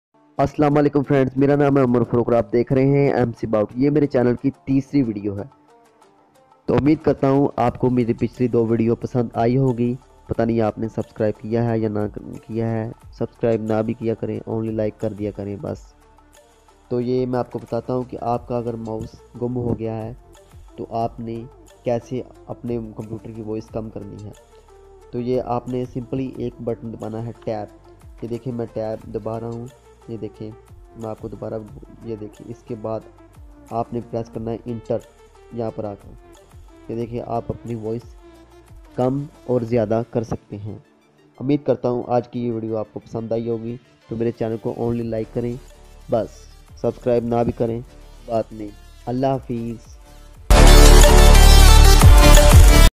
how to tab button use for shortcut ||| Amazing shortcuts ||| pc or laptop lecture ||| MC ABOUT
mc about
this is my three video.
In today's video I will show you how you can make voice more or less with the Tab button on the keyboard.
tab walay button ko kahan use krtay han,tab button ko kis function kay press krta han,tab button qu use kiya jata hai,
MC-ABOUT